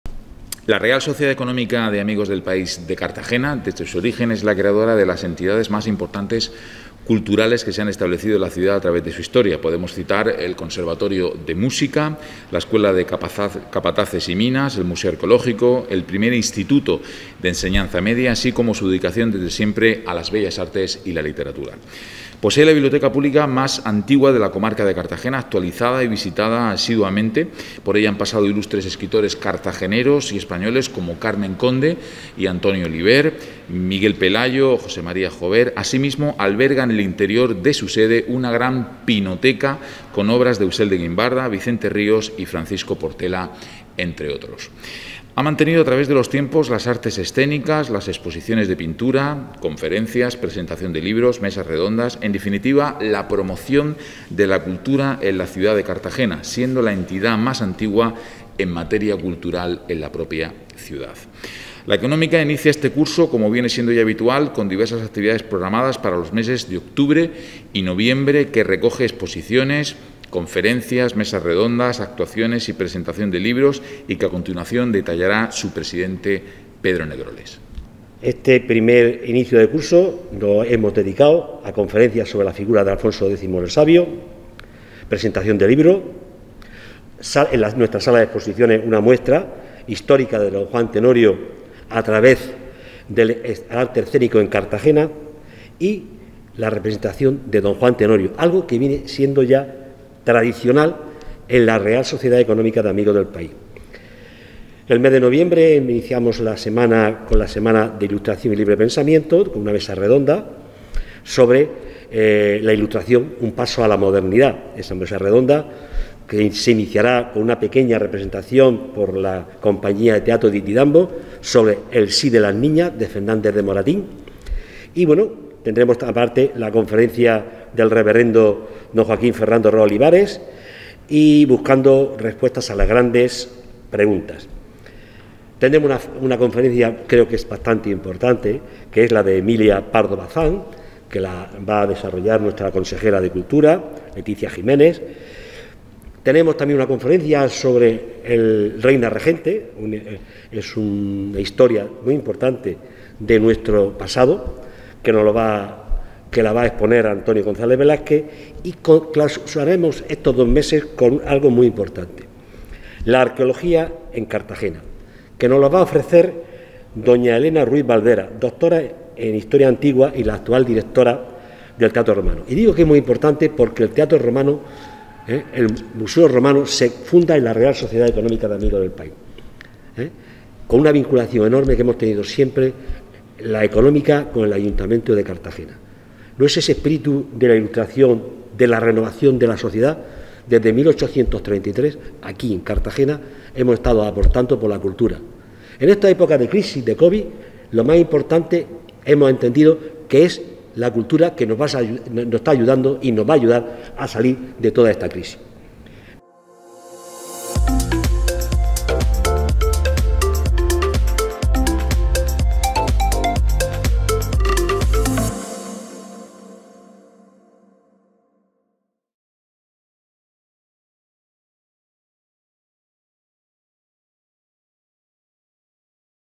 La Real Sociedad Económica de Amigos del País llevará el Tenorio a la calle del Aire
La Real Sociedad Económica de Amigos del País de Cartagena presenta su nuevo programa de actividades para los meses de octubre y noviembre. Pedro Negroles, presidente de esta institución, y Carlos Piñana, concejal delegado de Cultura, han expuesto los detalles de este ciclo cultural en el que colabora el Ayuntamiento de Cartagena.

Los cuatro actos programados para octubre concluirán con la representación de Don Juan Tenorio, que tendrá lugar el sábado 30 a las 20.30 horas en la calle del Aire, frente a la Real Sociedad Económica de Amigos del País. Del 25 al 30 de octubre también se podrá ver una exposición histórica sobre la presencia de este clásico de Zorrilla en la escena de Cartagena.

El jueves, día 7, habrá una conferencia sobre Alfonso X El Sabio, que correrá a cargo del cronista Francisco José Franco y el doctor en historia medieval José Marcos García. Será a las 20.00 horas. Mientras que el viernes, día 8, se presenta a la misma hora el libro ‘Desdicha y fortuna de Julianillo Valcárcel’, del escritor José Luis Abraham.

El mes de noviembre estará centrado en la celebración de las Jornadas de la Ilustración y el Libre Pensamiento. Del día 2 al 17 habrá conferencias sobre literatura, navegación, arqueología y espiritualidad. Estas jornadas comenzarán el día 2 de noviembre a las 19.00 horas con la representación de la obra ‘El Sí de las niñas’ y una mesa redonda sobre la Ilustración moderada por el propio edil Carlos Piñana.